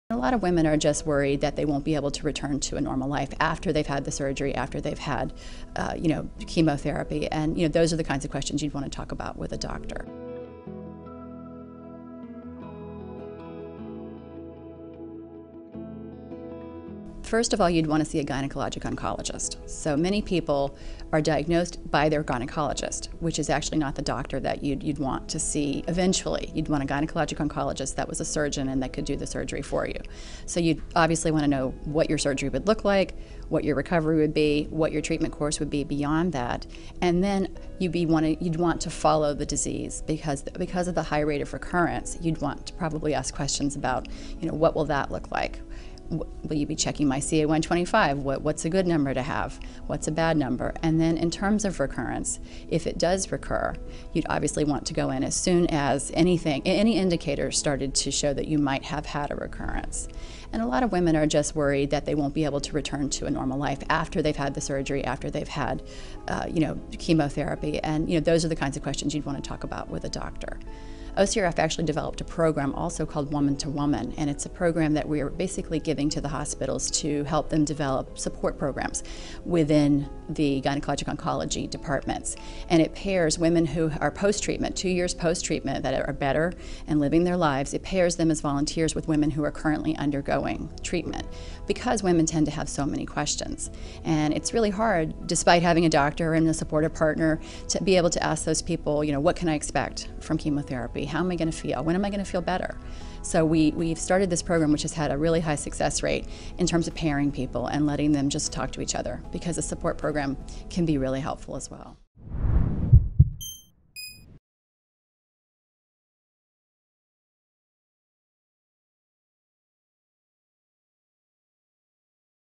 Ovarian Cancer: What to Know About the Disease | Sharecare
Audra Moran, CEO of the Ovarian Cancer Research Fund, understands the stress that's associated with being diagnosed with ovarian cancer. From understanding the procedure to recovery, ask these questions to make sure you're prepared for it all.

If I Have Been Diagnosed with Ovarian Cancer, What Questions Should I Ask My Doctor